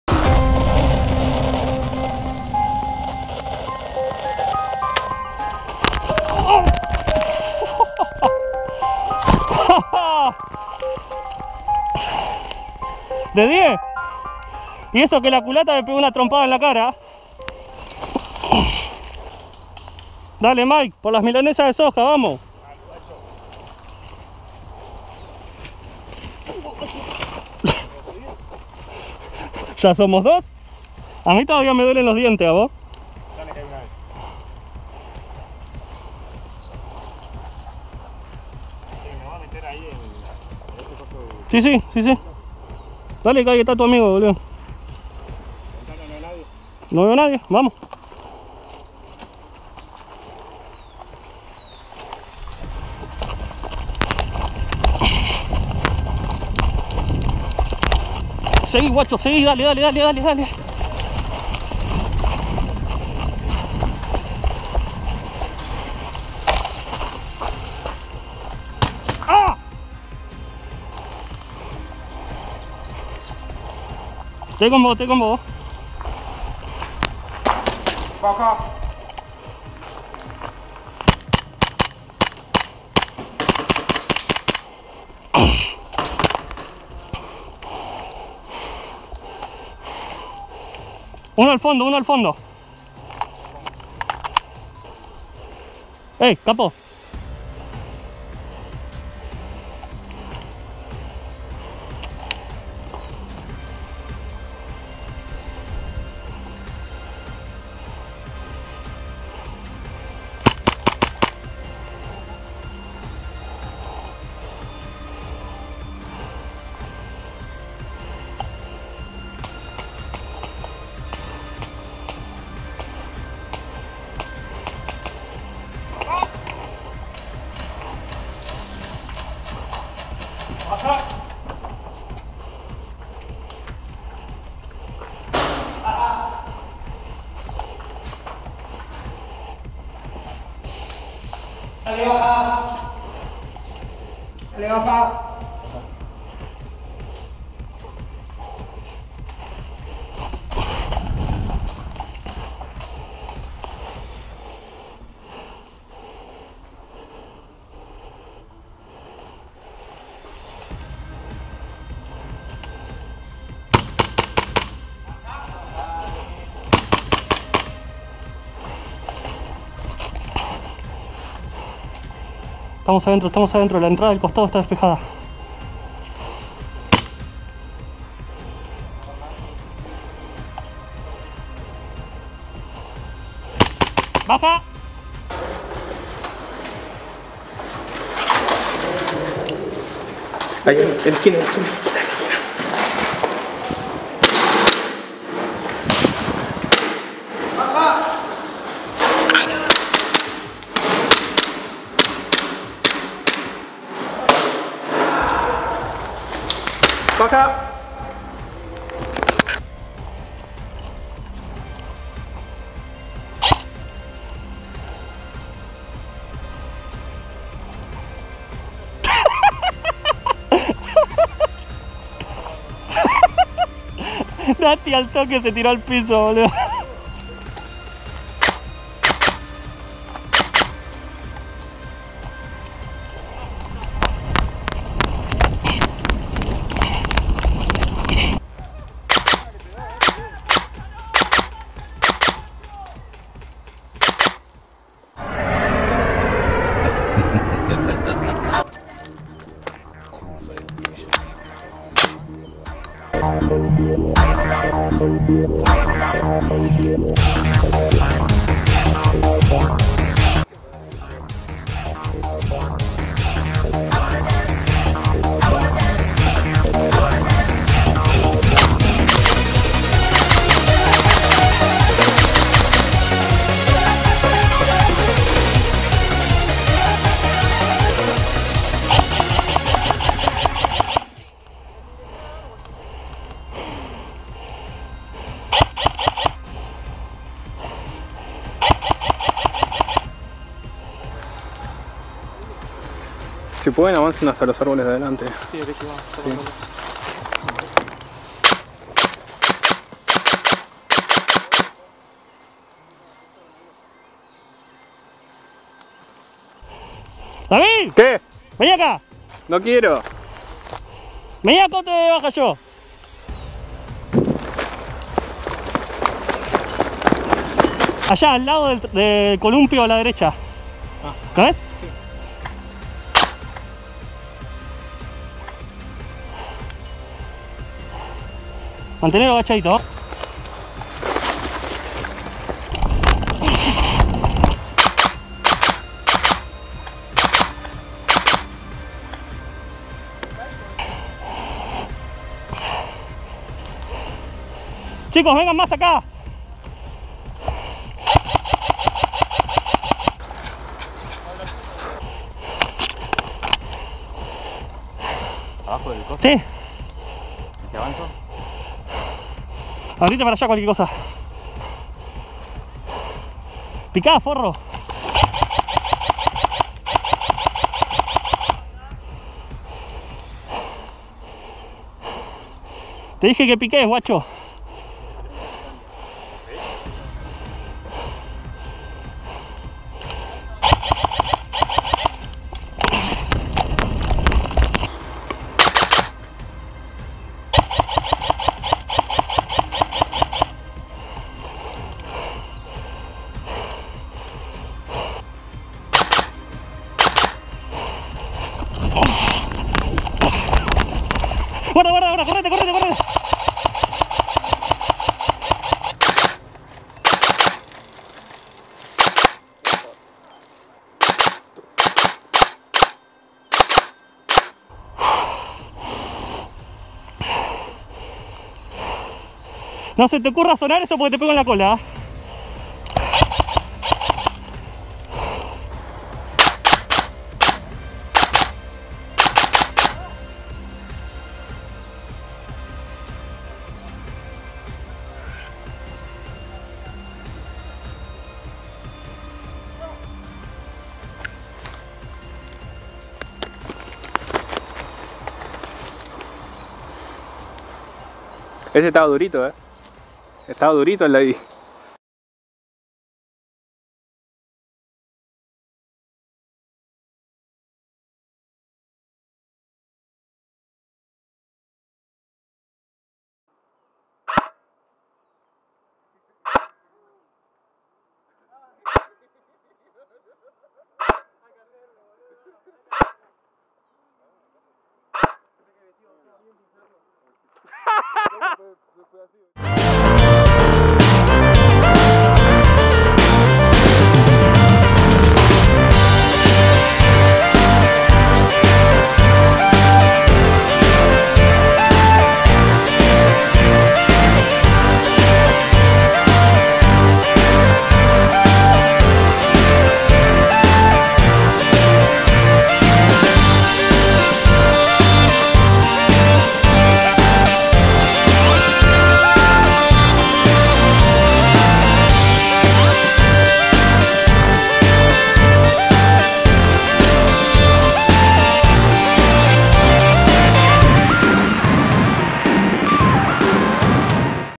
Airsoft Argentina Foxhound accidentado
Buenas Operators! hoy les dejo un compilado de la partida del pasado domingo en Foxhound, un juego bastante accidentado dado que el pasto estaba largo y tapaba los pozos.